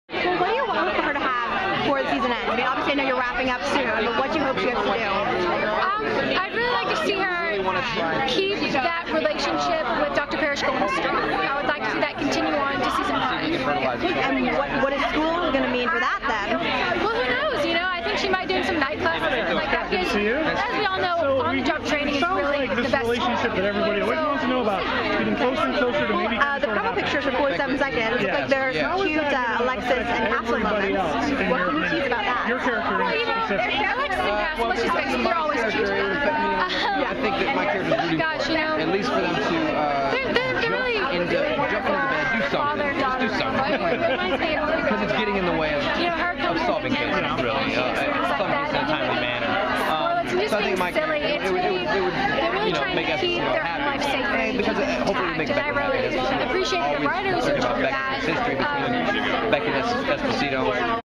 CASTLE: Molly Quinn Shares Her Hopes for the Rest of the Season
At PaleyFest 2012, CASTLE's Molly Quinn talks with GiveMeMyRemote.com's Marisa Roffman about her character.